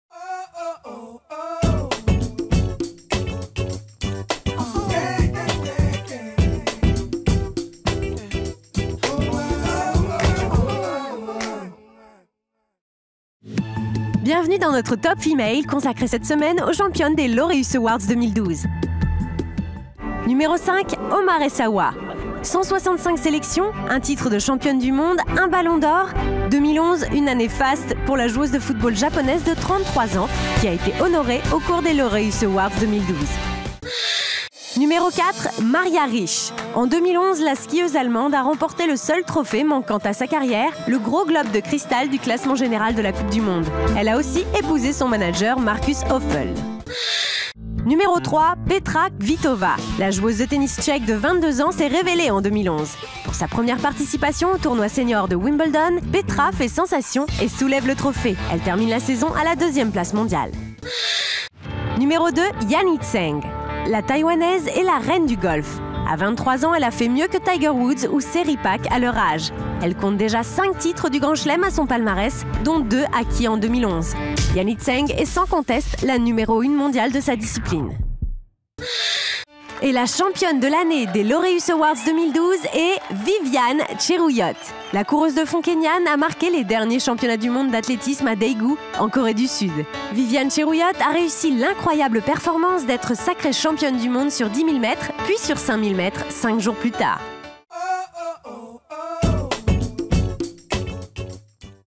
Top Female: Les gagnantes des Laureus Awards
Dans le top Female cette semaine retrouvez les lauréates des Laureus Awards.